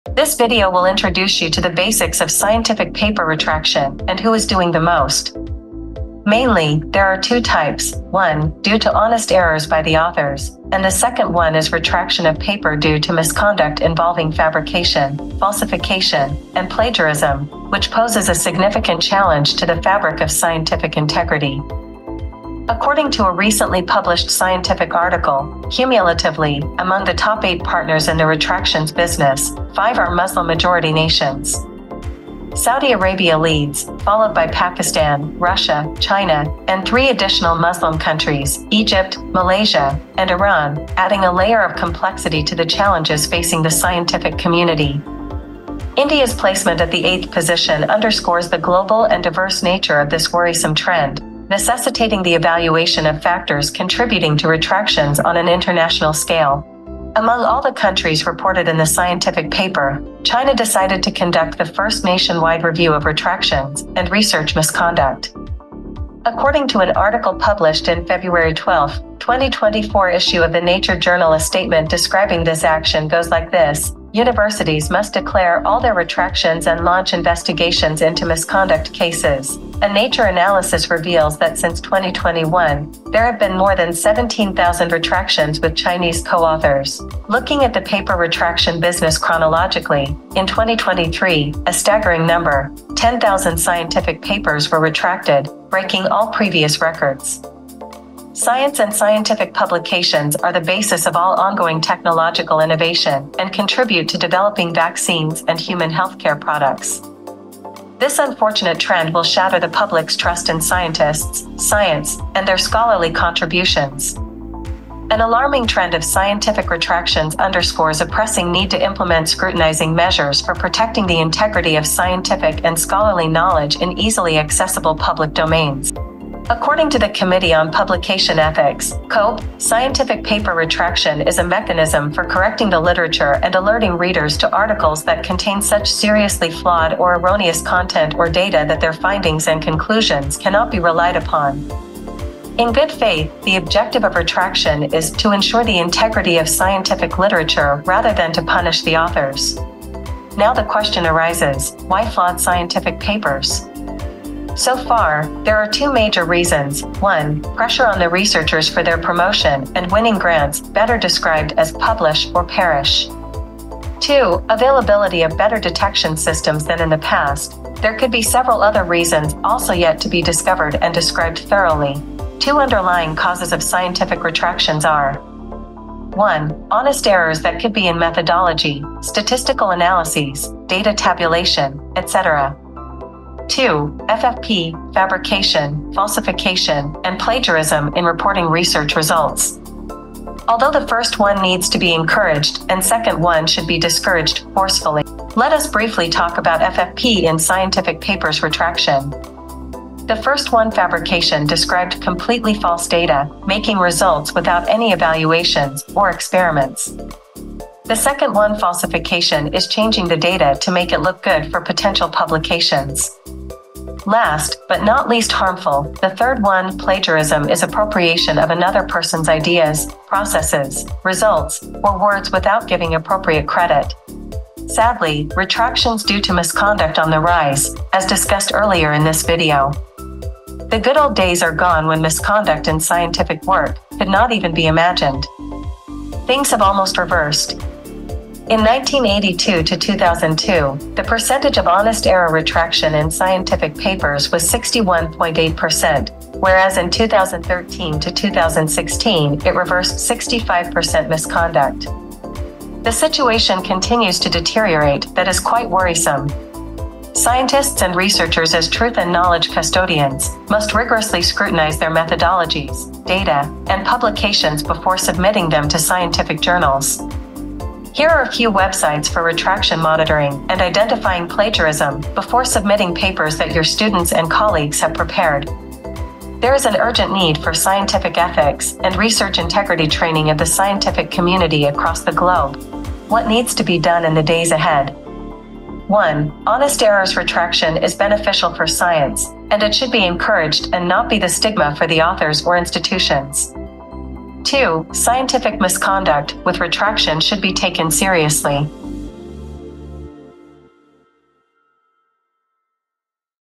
The Rising Tide of Scientific Paper Retractions - Threat to Researchers and Scientific Innovations
Introduction:
Scientific paper retractions have become a growing concern within the academic community, raising questions about the integrity of scientific research and the reliability of scholarly publications. This article aims to delve into the basics of scientific paper retractions, exploring the two main types and shedding light on the unsettling trend of misconduct leading to retractions, particularly in certain regions. Additionally, we will discuss the causes behind this rise and propose measures to address and prevent such incidents.

Types of Retractions:
Scientific paper retractions generally fall into two categories. The first is attributed to honest errors made by authors, encompassing mistakes in methodology, statistical analyses, data tabulation, and other unintentional discrepancies. The second type involves retractions due to misconduct, including fabrication, falsification, and plagiarism (FFP), which pose a severe threat to the fabric of scientific integrity.

Global Trends in Retractions:
A startling revelation in the realm of retractions is the significant involvement of Muslim-majority nations, with Saudi Arabia leading the pack, followed by Pakistan, Russia, China, Egypt, Malaysia, and Iran. This trend places a complex challenge before the scientific community, as the global landscape of retractions reflects diverse contributors. Notably, China has undertaken the first nationwide review of retractions, revealing over 17,000 retractions with Chinese co-authors since 2021.

The Escalation of Scientific Paper Retractions:
In 2023, 10,000 scientific papers were retracted, marking a new record and underscoring a troubling trend. This surge in retractions threatens to erode public trust in science and scholarly contributions, mainly as science plays a pivotal role in technological innovation and the development of healthcare products and vaccines.

Causes Behind Scientific Retractions:
The underlying causes of scientific retractions are categorized into honest errors and misconduct. The pressure on researchers for career advancement and grant acquisition, often summarized as "publish or perish," contributes to misconduct. The advancement in detection systems has also played a role in uncovering discrepancies. Meanwhile, FFP in reporting research results involving fabrication, falsification, and plagiarism remains a significant concern.

Addressing Fabrication, Falsification, and Plagiarism:
Fabrication involves the creation of entirely false data, while falsification entails manipulating data to present favorable results. Plagiarism, appropriating someone else's ideas without proper credit, poses a serious ethical breach. To curb these practices, a collective effort is needed, including robust ethical training for researchers, rigorous scrutiny of methodologies, and the use of plagiarism detection tools.

The Alarming Shift in Trends:
Historically, retractions were predominantly due to honest errors, accounting for 61.8% from 1982 to 2002. However, from 2013 to 2016, misconduct-related retractions soared to 65%, signaling a worrisome shift. This underscores the urgency for scientists and researchers to review their work before submission meticulously.

A Call for Ethical Vigilance:
In light of the escalating trend in retractions, there is an urgent need for ethical vigilance and research integrity training across the global scientific community. Scientists and researchers must use websites for retraction monitoring, and plagiarism detection should be actively utilized to ensure the credibility of scientific publications. Honest error retractions should be encouraged to improve scientific literature without stigmatizing authors or institutions.

Conclusion:
The rising tide of scientific paper retractions, particularly those driven by misconduct, poses a significant threat to the integrity of scholarly knowledge. The scientific community must unite in its commitment to ethical conduct, transparent research practices, and the continuous improvement of scientific literature. By addressing the root causes and implementing preventive measures, we can safeguard the trust placed in scientists, science, and the invaluable contributions made to our collective knowledge.